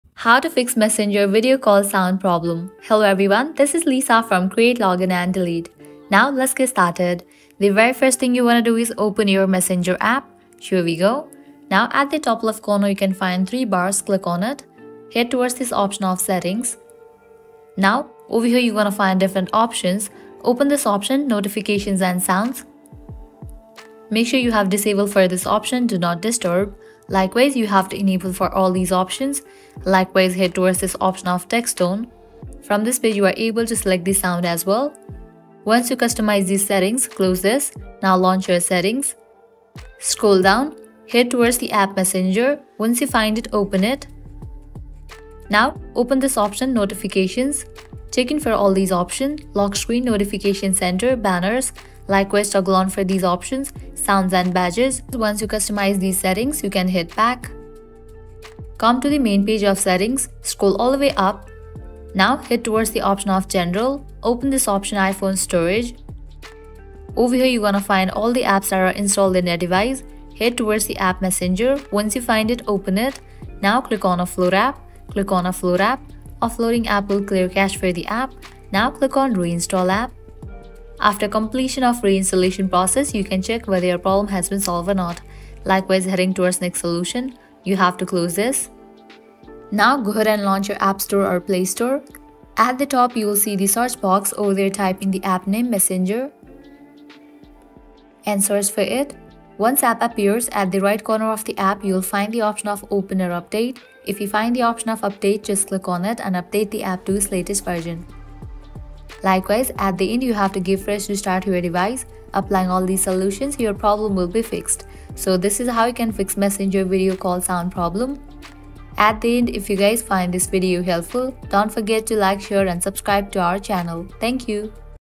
How To Fix Messenger Video Call Sound Problem 2024 | Audio Disabled In Messenger Call Fix
Tutorial On How To Fix Messenger Video Call Sound Problem 2024.

Welcome to Create Login And Delete! Are you facing audio issues during Messenger video calls in 2024? Whether you're using an iPhone or an Android device, this comprehensive guide is designed to help you troubleshoot and fix sound problems in your Messenger video calls.

Join us as we explore the solutions to ensure clear and uninterrupted audio in your Messenger calls.

🔑 Topics Covered:

How To Fix Messenger Video Call Sound Problem 2024: We provide a general guide for troubleshooting audio issues in Messenger video calls, applicable to all devices and updated with the latest 2024 information.

How to Fix Messenger Video Call Sound Problem on iPhone: Specific steps and tips for iPhone users to resolve sound problems during Messenger video calls, ensuring smooth communication on iOS devices.

How to Fix Messenger Video Call Sound Problem on Android: Detailed solutions for Android users to tackle audio issues in Messenger calls, tailored for the Android operating system.

🕒 Timestamps for Easy Navigation:

0:00 - Introduction
0:30 - General Troubleshooting for Messenger Audio Issues
1:00 - Fixing Sound Problems on iPhone
1:30 - Resolving Audio Issues on Android

This guide is ideal for anyone experiencing sound problems in Messenger video calls. Any questions or need more assistance?